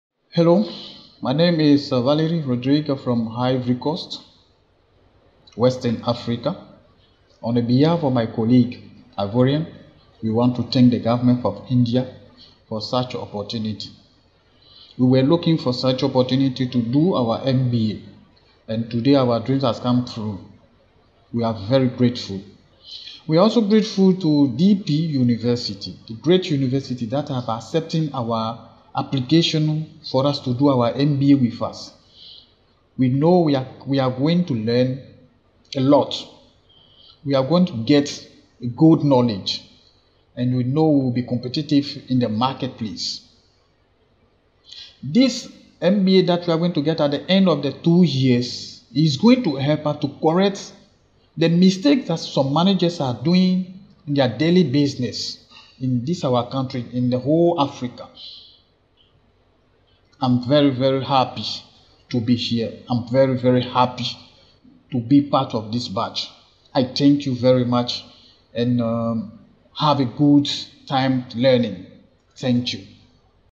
StudentSpeak - Mr. Valery Goulehi Rodrigue, DPUCOL Online MBA Degree Program, Africa!
"I am very grateful to DPUCOL which is accepting our application to do an MBA. I am hopeful that I am going to learn a lot by gaining good knowledge and will be competitive in the marketplace," shares Valerie Rodriguez from Western Africa.